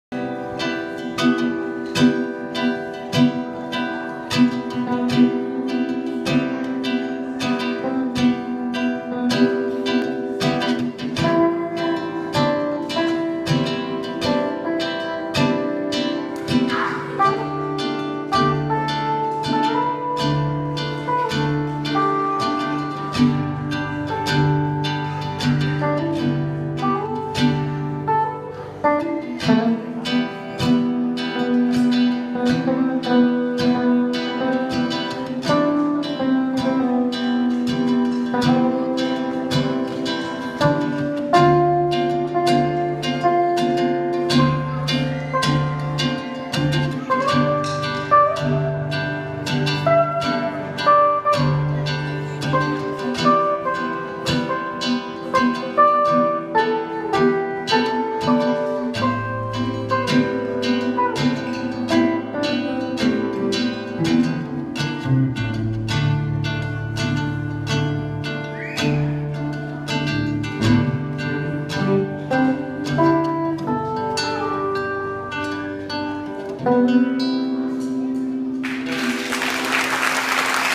2a Audição da escola de música Central Music Mogi Mirim
Gustavo Brandino com a professora Tainá no improviso!